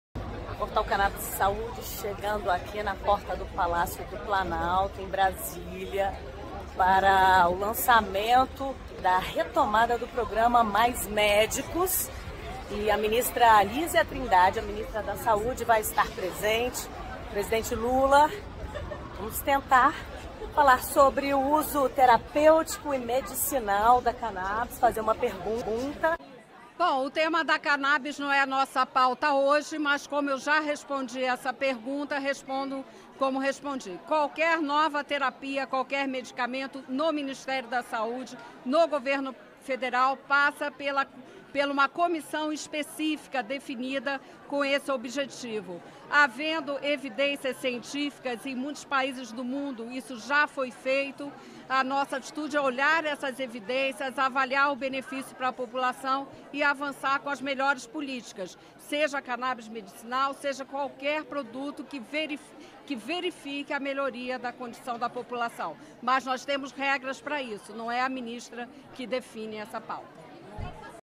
Entrevista com a Ministra da Saúde, Nísia Trindade
Hoje o Portal Cannabis e Saúde acompanhou de perto o anuncio da retomada do programa Mais Médico, e fez uma pergunta a Ministra da Saúde, Nísia Trindade se há espaço nesse governo para um Programa Nacional de Fornecimento de produtos à base de Cannabis via SUS.